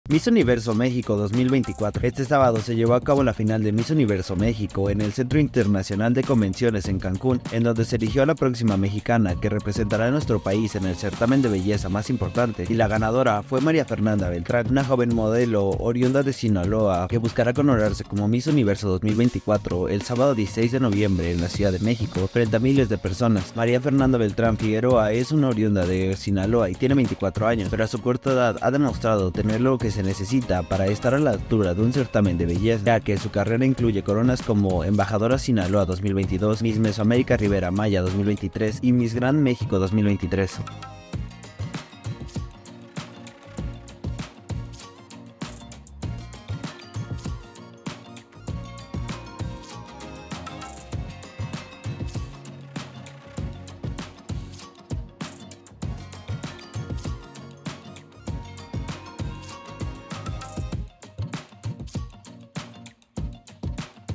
Miss Universo México 2024: Ella es María Fernanda Beltrán, la ganadora sinaloense
Este sábado se llevó a cabo la final de Miss Universo México en el Centro Internacional de Convenciones en Cancún en donde se eligió a la próxima mexicana que representará a nuestro país en el certamen de belleza más importante